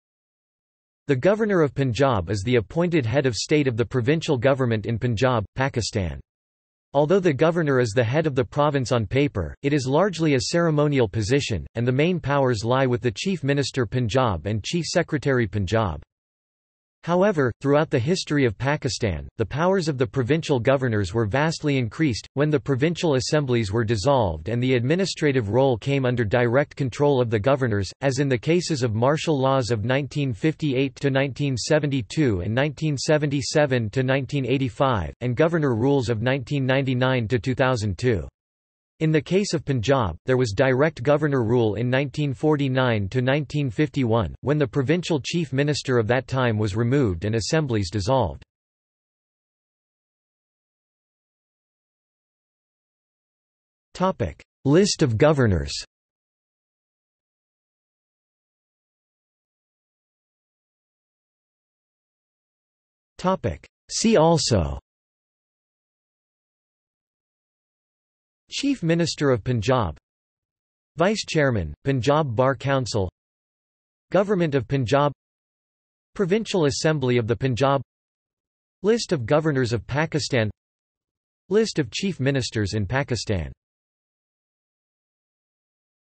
Governor of Punjab, Pakistan | Wikipedia audio article
This is an audio version of the Wikipedia Article:
Governor of Punjab, Pakistan

00:00:50 undefined
00:00:59 undefined

- Socrates

SUMMARY
The Governor of Punjab is the appointed head of state of the provincial government in Punjab, Pakistan. Although the Governor is the head of the province on paper, it is largely a ceremonial position; and the main powers lie with the Chief Minister Punjab and Chief Secretary Punjab.
However, throughout the history of Pakistan, the powers of the provincial governors were vastly increased, when the provincial assemblies were dissolved and the administrative role came under direct control of the governors, as in the cases of martial laws of 1958–1972 and 1977–1985, and governor rules of 1999–2002. In the case of Punjab, there was direct governor rule in 1949–1951, when the provincial chief minister of that time was removed and assemblies dissolved.